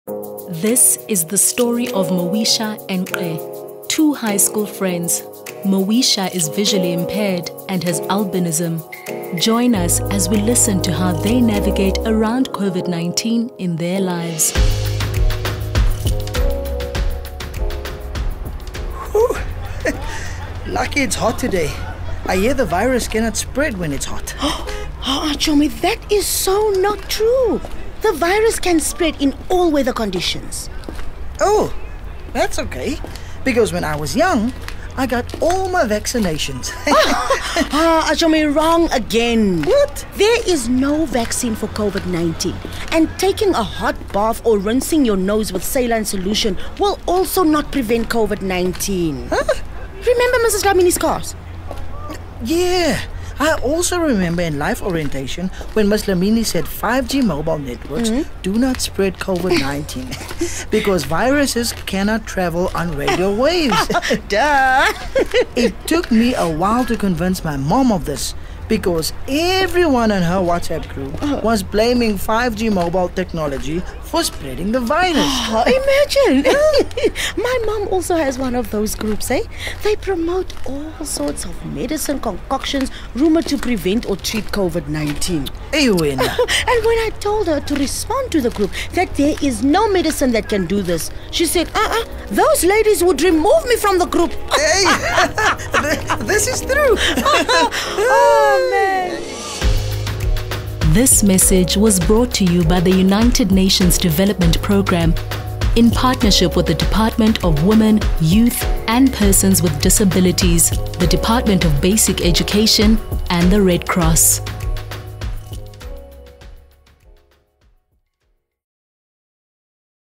CORONA O'CLOCK - Theme: Myth Busting
The United Nations Development Programme launched Corona O’clock, a comic expressing the COVID-19 experiences of Moesha, a visually impaired teenager with albinism and her friend !Ke. Messages in the comic talk to different aspects of COVID-19 including personal hygiene, physical distancing, knowing the symptoms, kindness contagion, myth busting, doing more and building back better.

Supported by the Government of Japan, this initiative aims to influence the perceptions and behaviours of adolescents, thus reducing their risk of contracting COVID-19. It addresses seven themes - personal hygiene, physical distancing, knowing the symptoms, kindness contagion, myth busting, doing more and building back better.